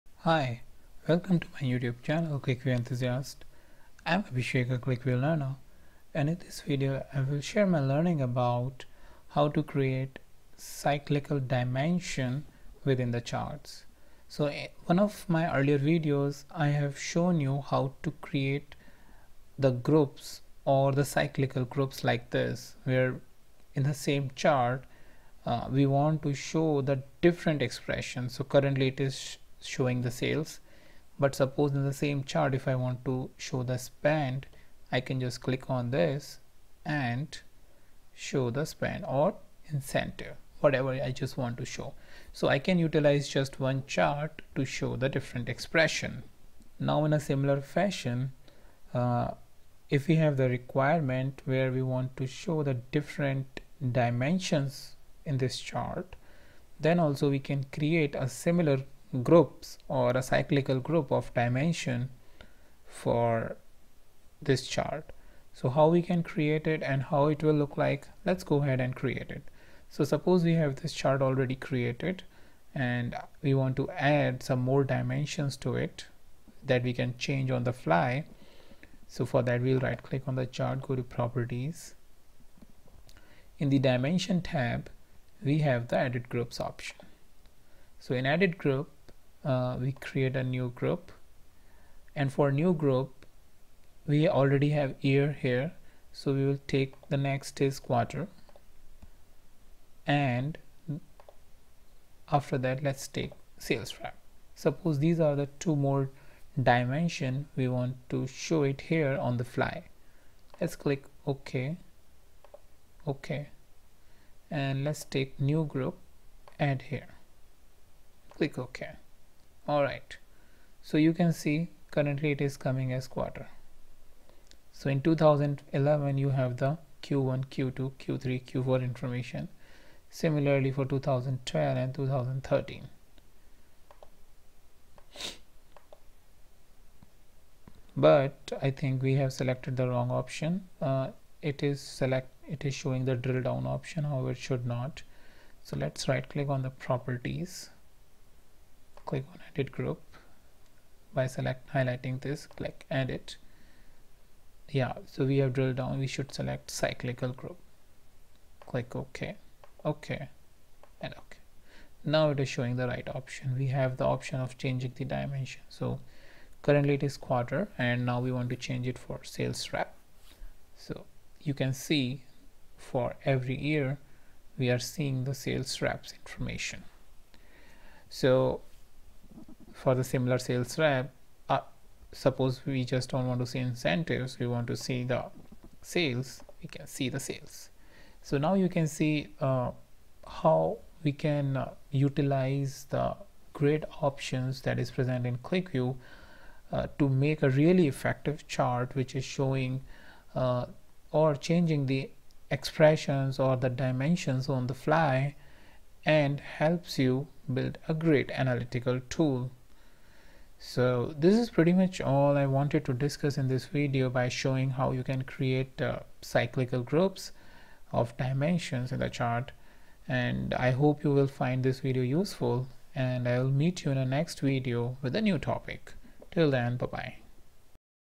QlikView Tutorial | QlikView Expressions | QlikView Cyclic Dimension or Group Dimension
In this video I've talked about how to use the Cyclic dimension feature which is very similar to grouping expression video which I last posted. Here in this video I showed how you'll create the Cyclic Dimension or Group dimension to create an analytical chart.

I hope you'll find this video useful and an addition to your existing learning of QlikView.

-Abhishek